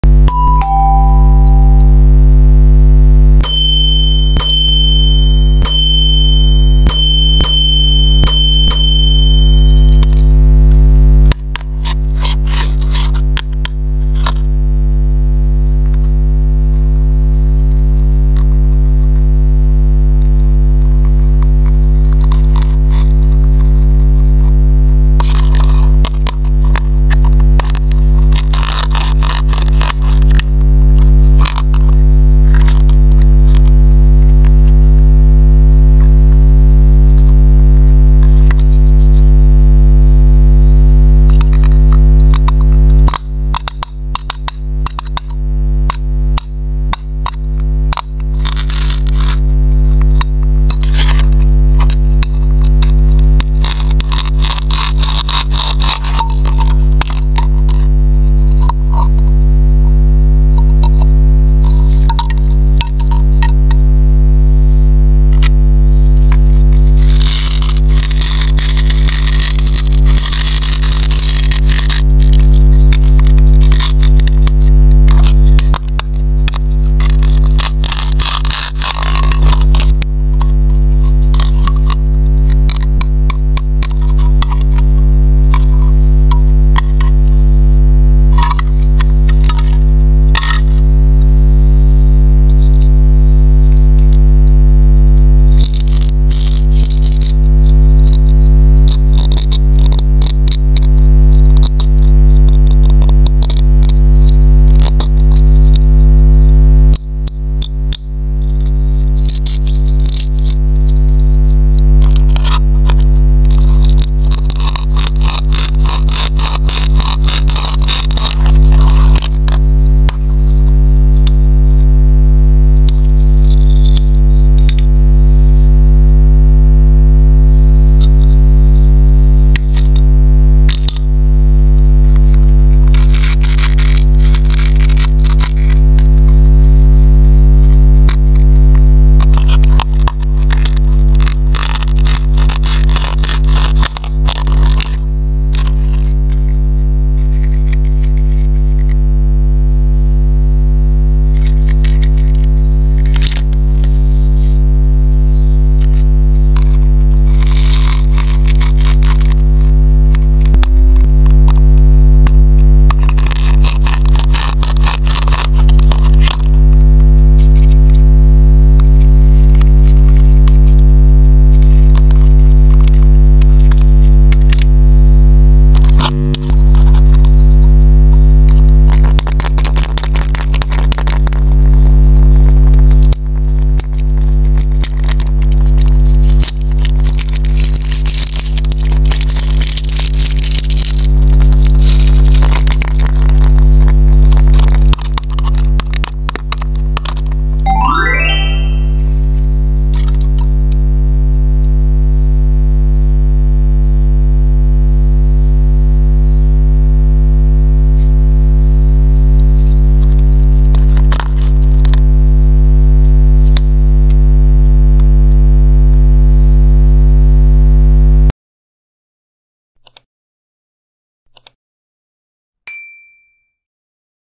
Fruit ASMR Satisfying |how to cut Fruits vegetable toy ASMR video. sound cut noice 3 minutes
Satisfying Relaxing video ASMR.  I cutting plastic fruits .
I cut vegetables and fruits every day .